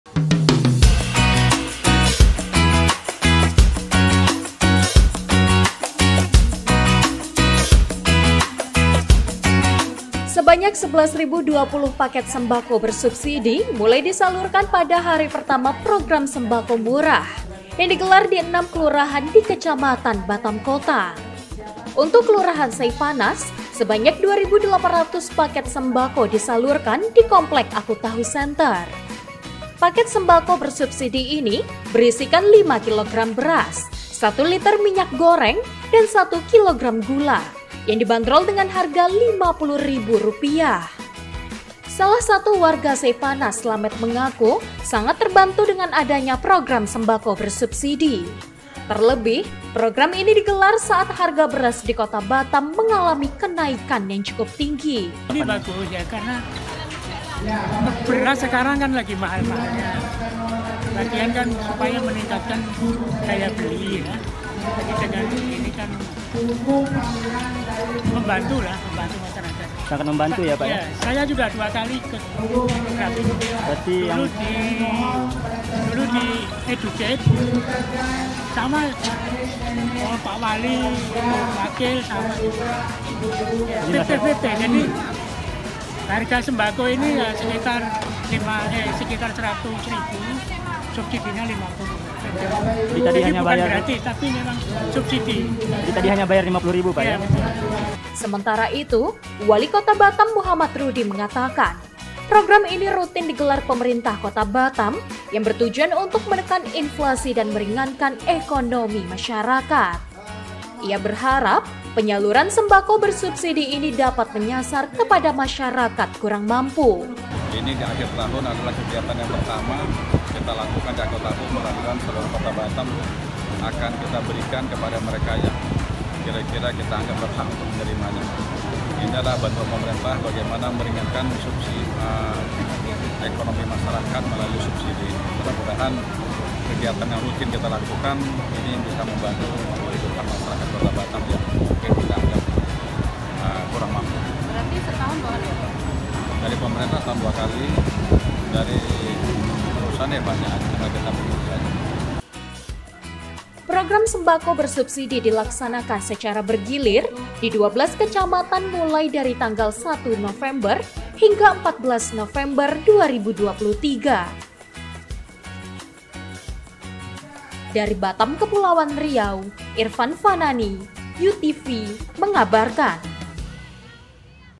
11.020 PAKET SEMBAKO SUBSIDI DISALURKAN DI KECAMATAN BATAM KOTA | U-NEWS
BATAM - Pemerintah Kota Batam mendistribusikan sebanyak 11.020 paket sembako bersubsidi, pada hari pertama pelaksanaan program sembako murah, yang dilaksanakan di enam kelurahan yang ada di kecamatan Batam Kota

Paket sembako bersubsidi ini berisikan 5 kilogram beras, 1 liter minyak goreng dan 1 kilogram gula yang dibanderol dengan harga 50 ribu rupiah.

Program sembako bersubsidi dilaksanakan secara bergilir di 12 kecamatan mulai 1 hingga 14 November 2023

Berikut Liputannya.

Produser : Suhardi